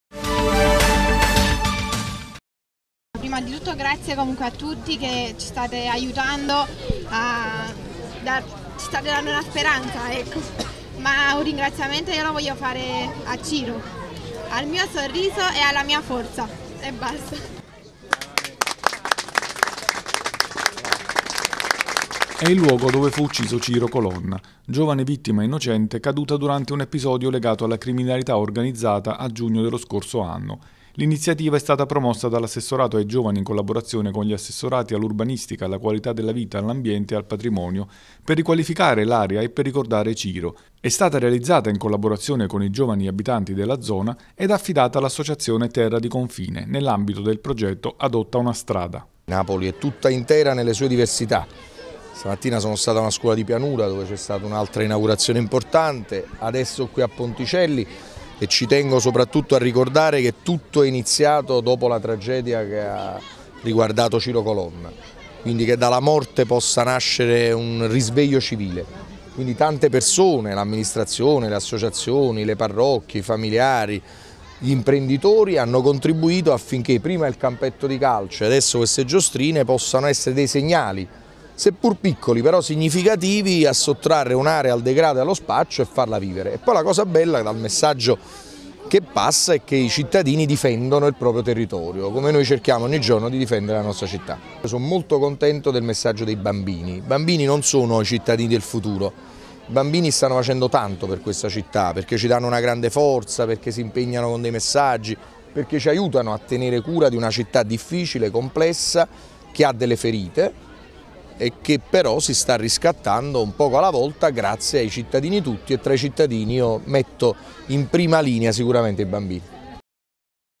Un'area giochi in ricordo di Ciro Colonna
Nel lotto O di Ponticelli nasce uno spazio per i giochi: è il luogo dove fu ucciso lo scorso anno Ciro Colonna giovane vittima innocente della criminalità organizzata.